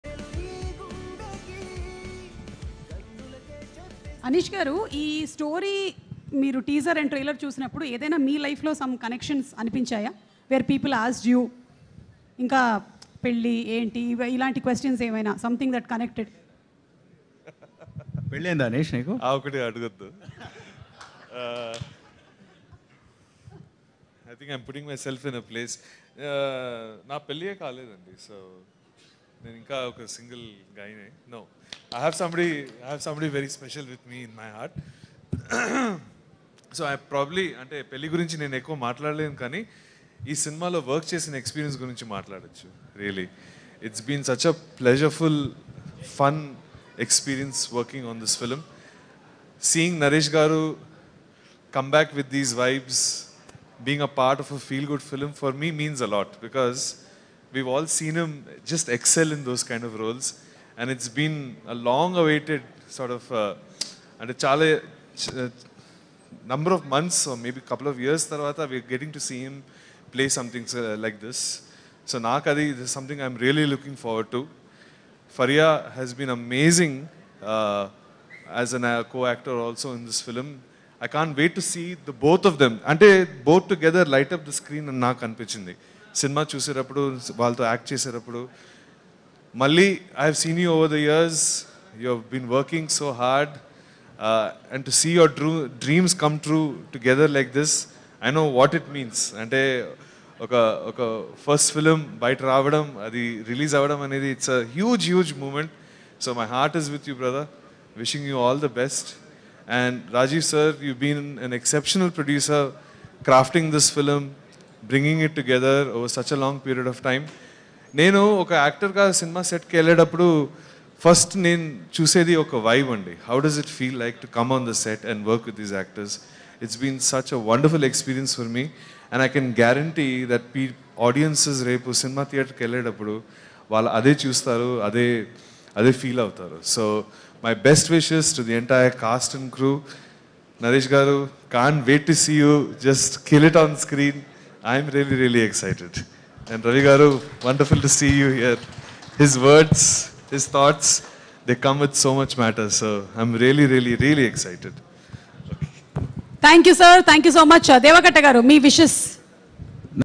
చాలా రోజుల తర్వాత కామెడీ సినిమాలో నరేష్ | Anish Kuruvilla Speech Aa Okkati Adakku Pre Release Event
Aa Okkati Adakku Pre Release Event LIVE | Allari Naresh | Faria Abdullah | Gopi Sundar | Vanitha Tv